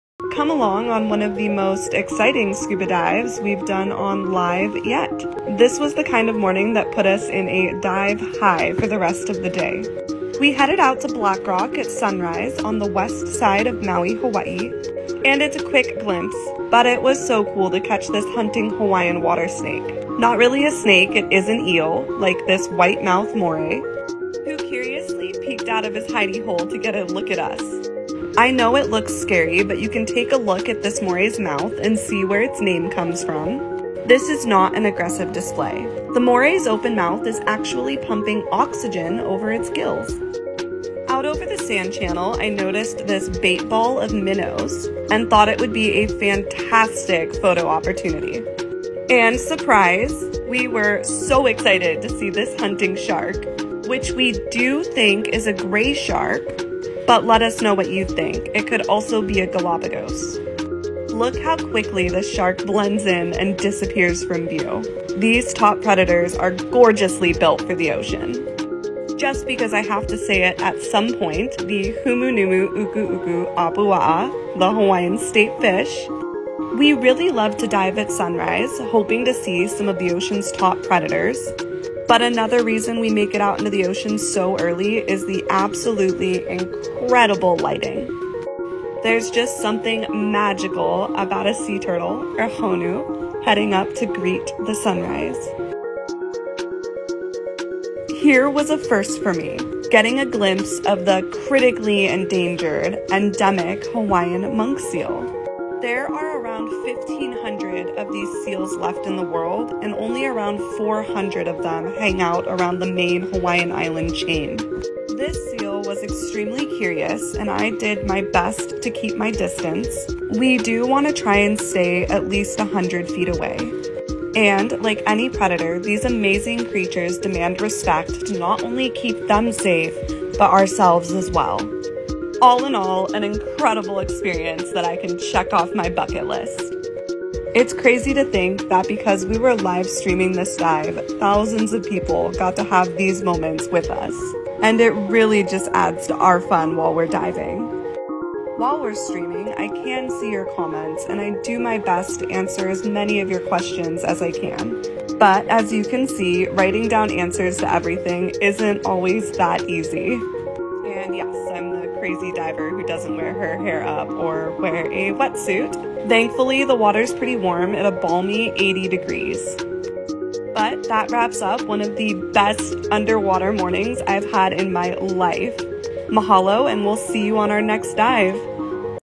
scubadiving Come along on one of our most memorable morning dives yet! We were
scubadiving Come along on one of our most memorable morning dives yet! We were live streaming this dive at Black Rock, Ka’anapali, Maui- and this is the footage we caught 殺 let us know your favorite moment!!! comedivewithus merlife sharks monkseal hawaii scuba ocean LearnOnTikTok #